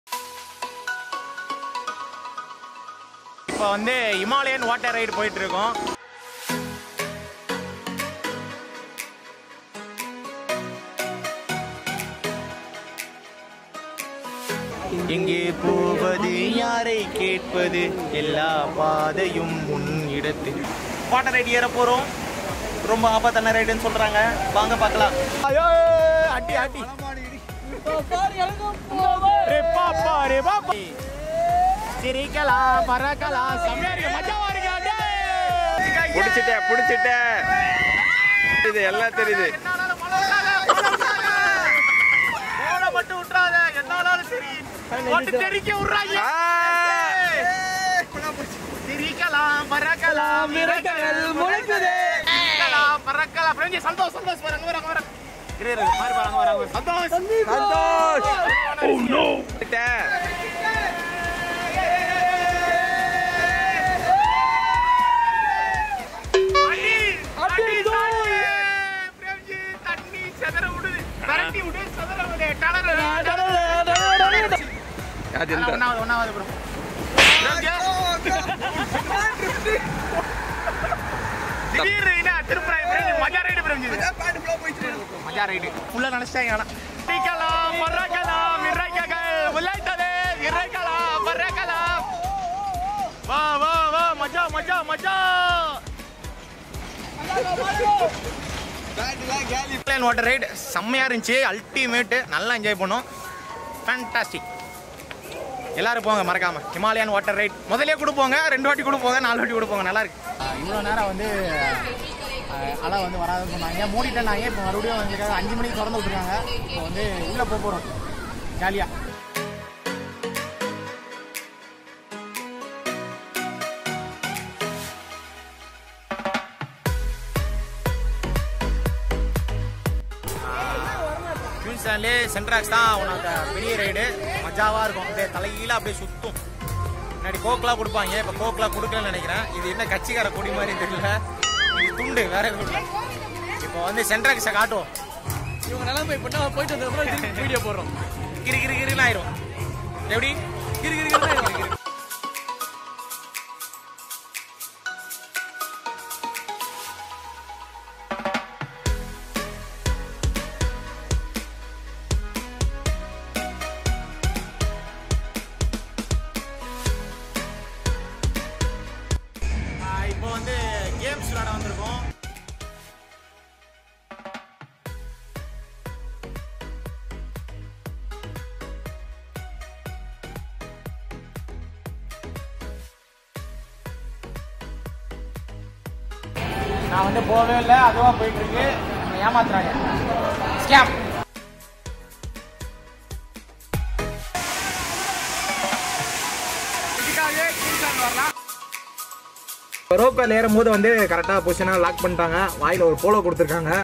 Himalayan Water Ride | Queensland | Amusement Park | Part 2 | Chennai | Theme Park | Assault Pasanga
Queensland Vlog part -2

Queens Land is a theme park in Poonamallee, Chennai, India, covering 70 acres. It opened in August 2003.